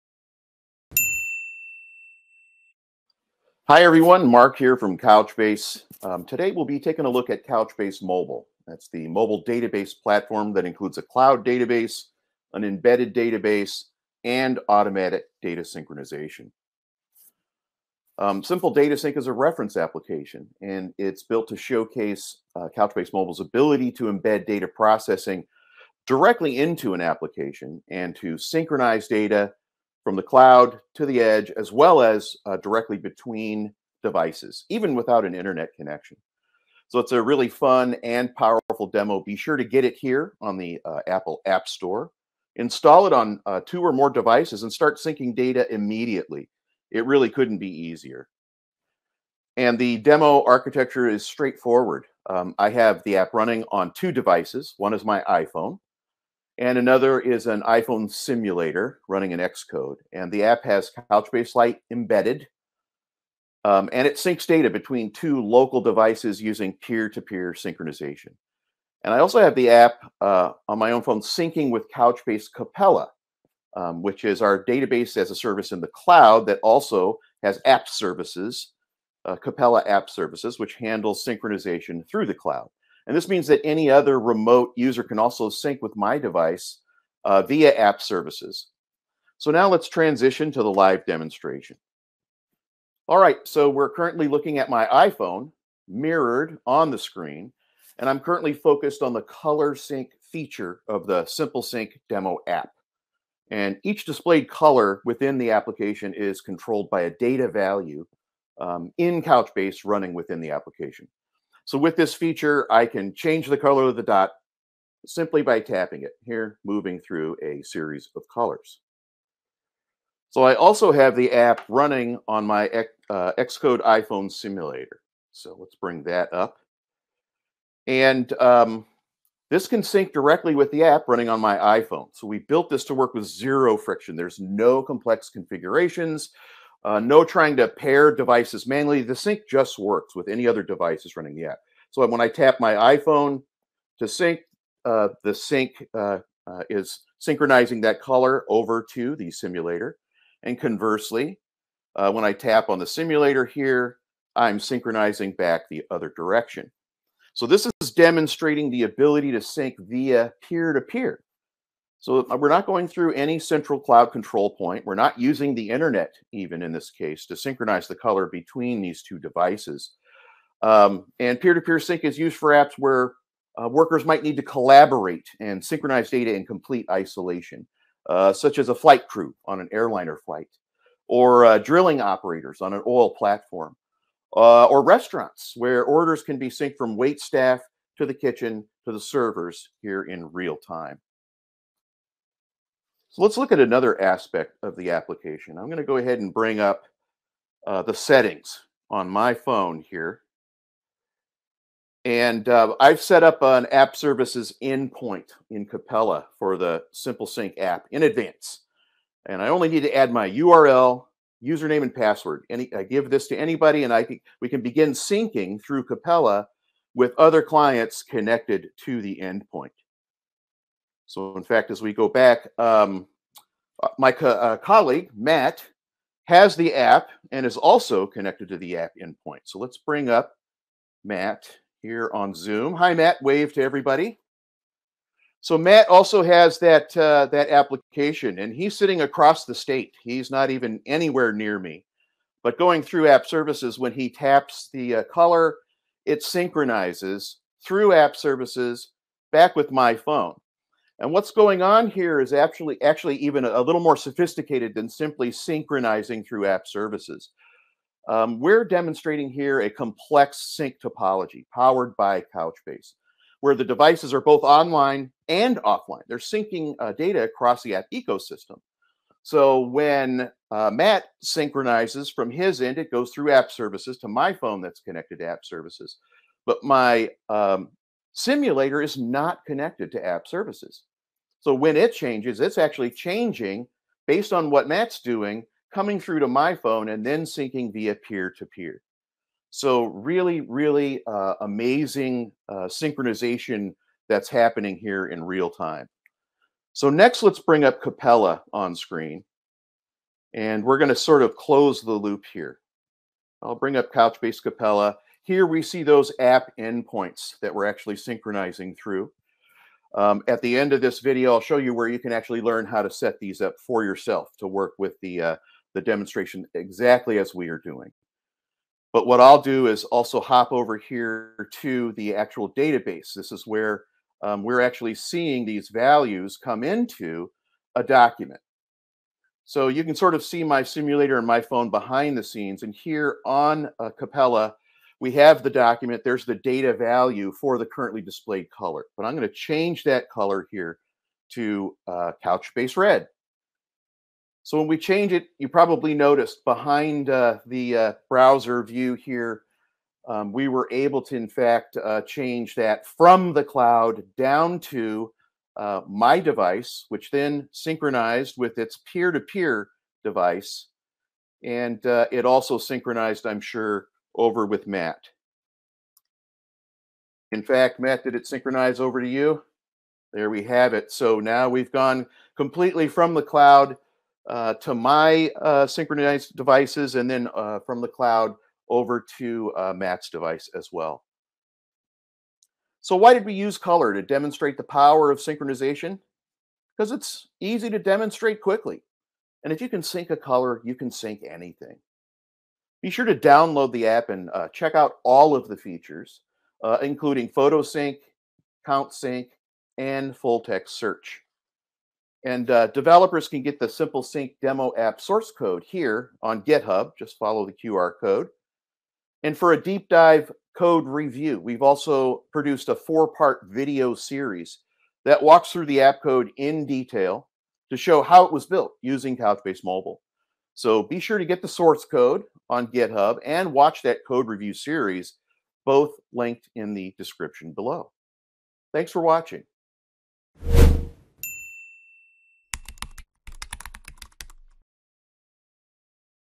Couchbase Mobile - live data sync demo
See cloud-to-edge and device-to-device mobile data sync in action using Couchbase Mobile, the mobile database platform that includes a cloud database, an embedded database and automatic data sync. Couchbase Mobile enables you to embed data processing directly into an application, and to synchronize data from the cloud to the edge, and between devices even without an internet connection.